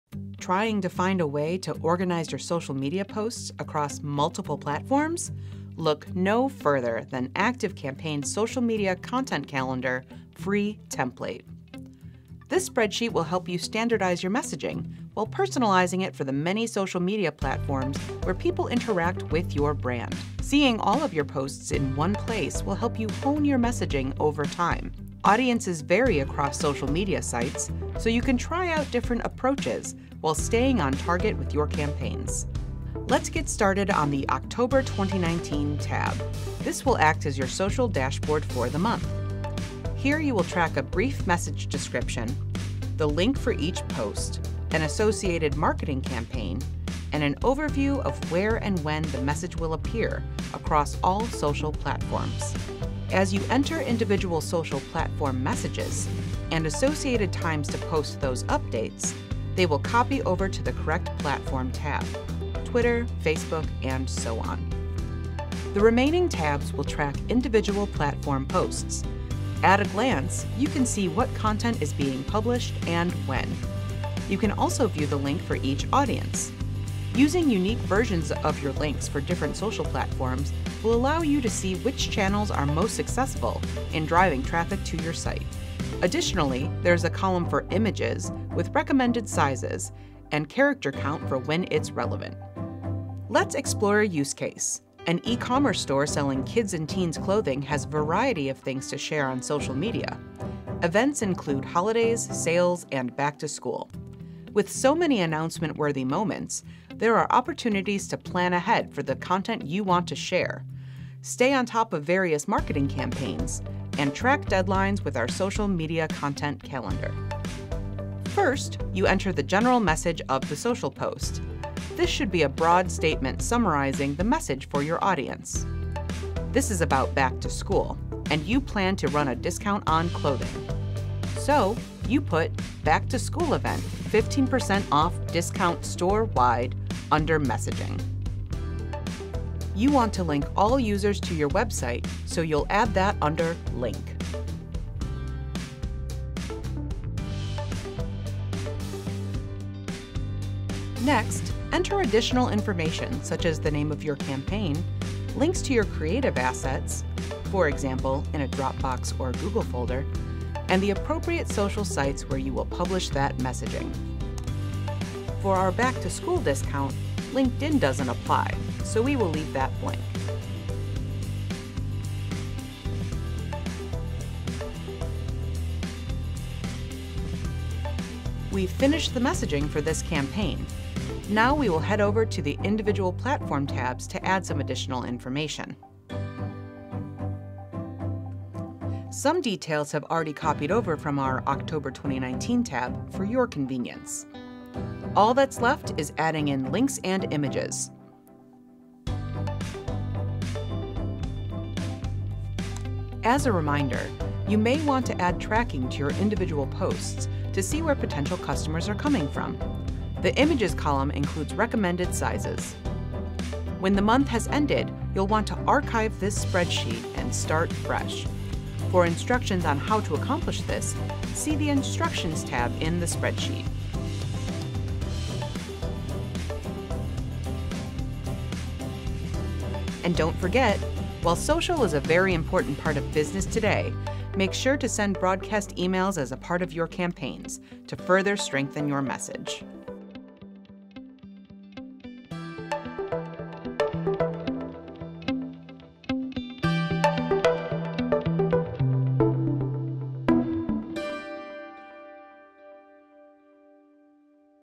How to use ActiveCampaign's free Social Media Calendar Template for Google Sheets & Excel
With this template you will better plan ahead what content to share, manage your marketing campaigns, track deadlines, and ultimately build stronger relationships.

Get started on the "Oct 2019" tab -- this will act as your social dashboard for the month. Here you will track a brief message description, the link for each post, an associated marketing campaign, and an overview of where and when the message will appear across all social platforms. As you enter individual social platform messages and associated times to post those updates–they will copy over to the correct platform tab (eg Twitter, Facebook, and etc.).

The remaining tabs will track individual platform posts. At a glance, see what content is being published and when. You can also view the link for each audience. Additionally, there is a column for images (with recommended sizes) and character count for when it's relevant (eg. Twitter and Pinterest).

Get started by watching this walkthrough (with an example) of how to use our free Social Media Content Calendar template for both Excel and Google Sheets.